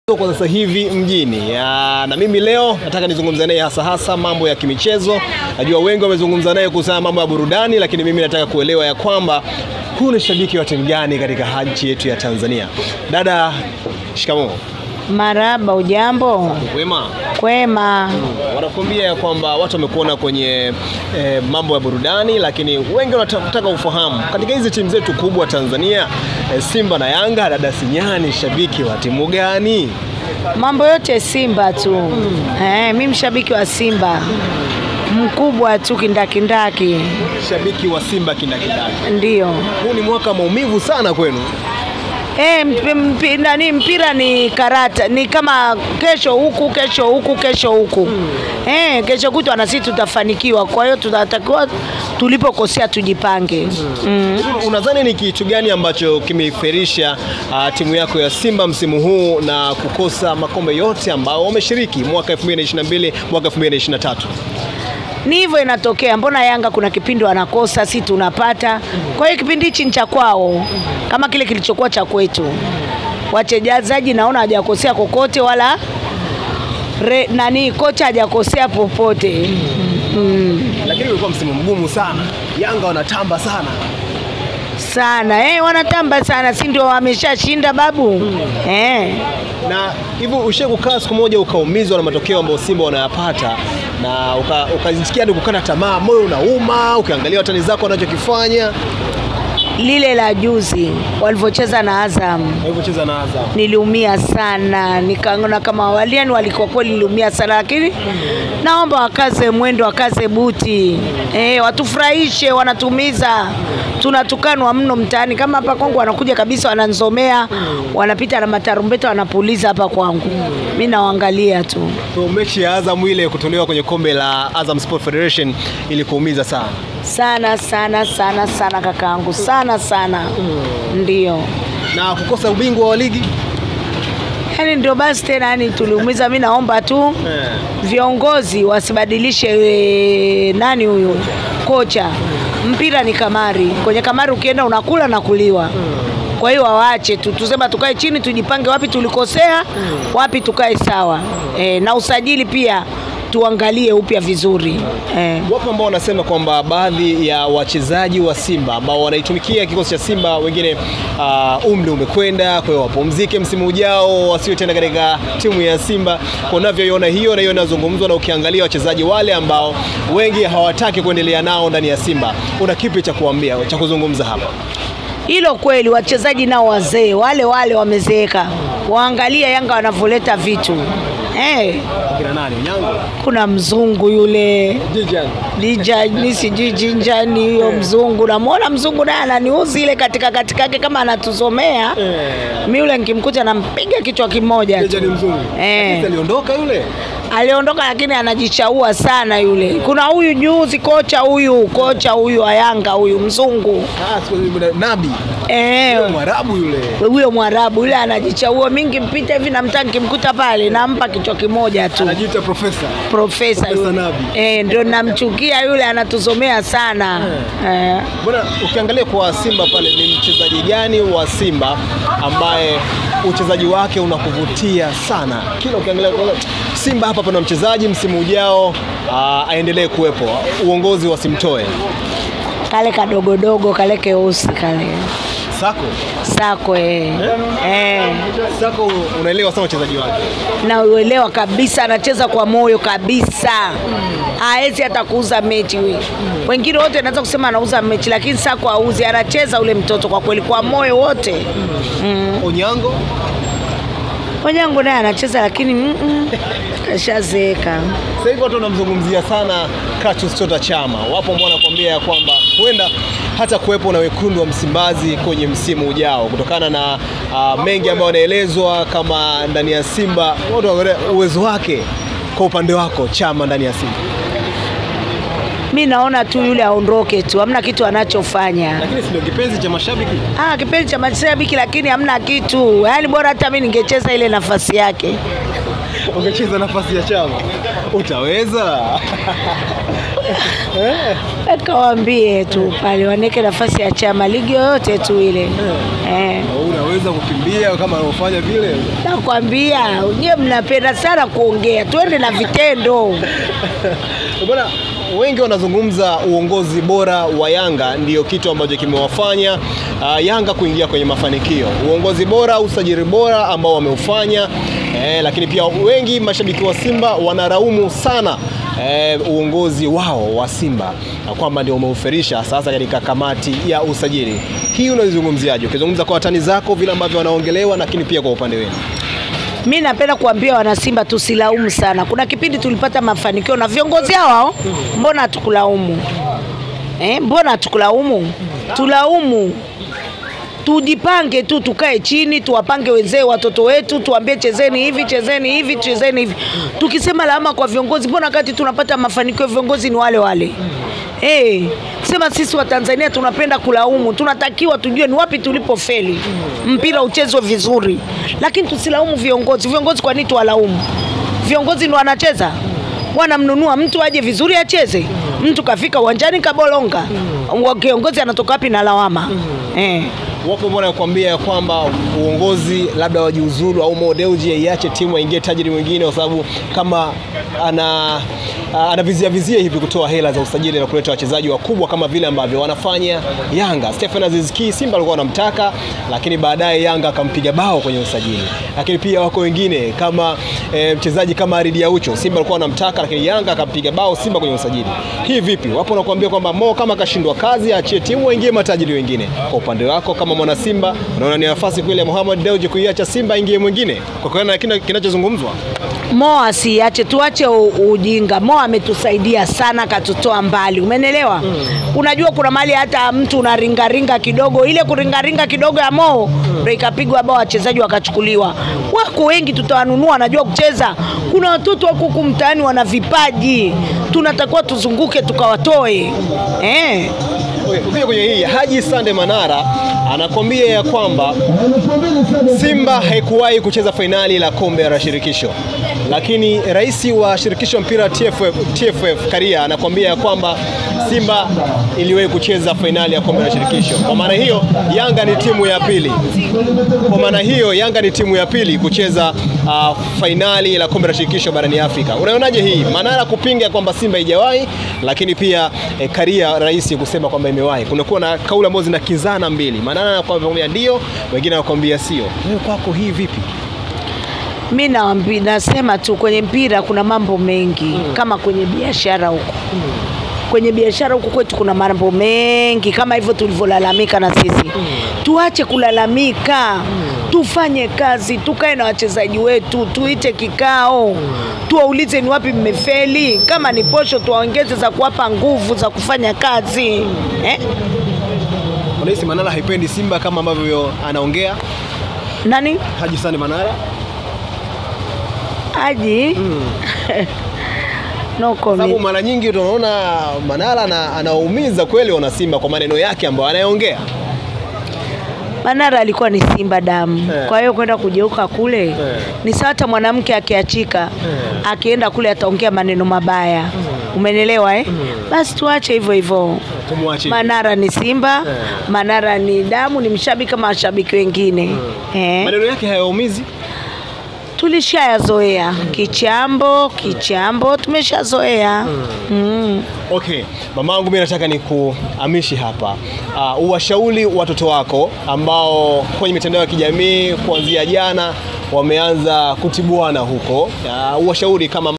KOCHA MGUNDA ALITAMANI TOKA MUDA KUONDOKA SIMBA/SINYALI TUNAPATA AIBU MASHABIKI/AHMED ALI ATOA NENO
Mimbayazuchu yazua utata ukweni kaka wa zuchu afichua harusidiamond wazae baada ya ndoa
Tufollow katika ukurasa wetu wa instagram  @PLANTAMEDIA
Na kulike facebook page planta media